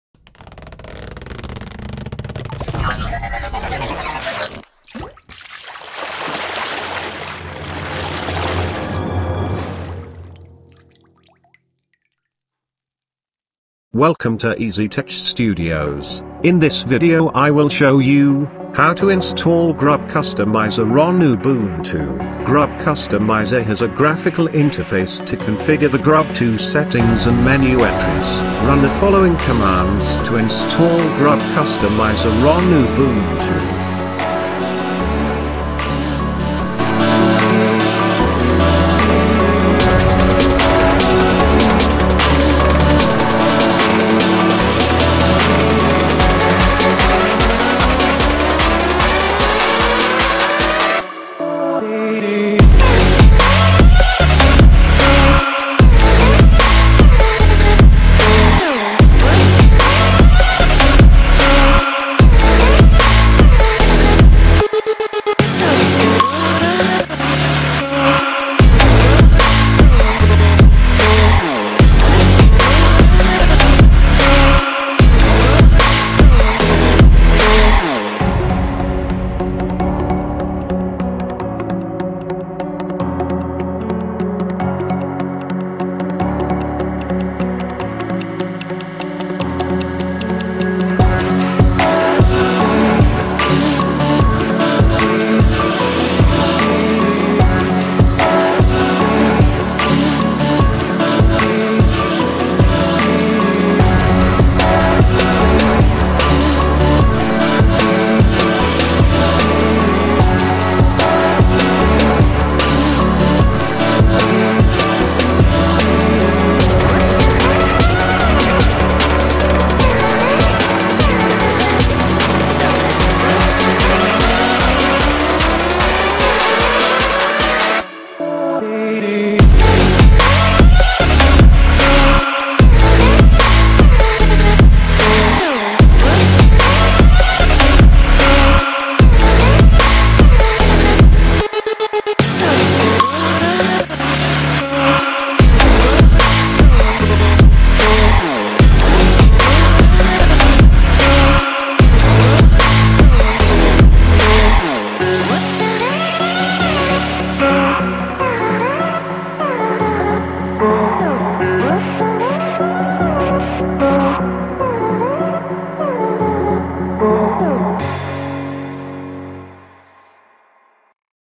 How To Install Grub Customizer on Ubuntu 16.04 | 17.10
Grub Customizer has a graphical interface to configure the GRUB2/BURG settings and menu entries.

Run the following commands to install Grub Customizer on Ubuntu:
sudo add-apt-repository ppa:danielrichter2007/grub-customizer
sudo apt-get update
sudo apt-get install grub-customizer

Learn online IT and software courses.

If you want to uninstall and remove the Grub Customizer from Ubuntu, run the command given below:
sudo apt-get remove grub-customizer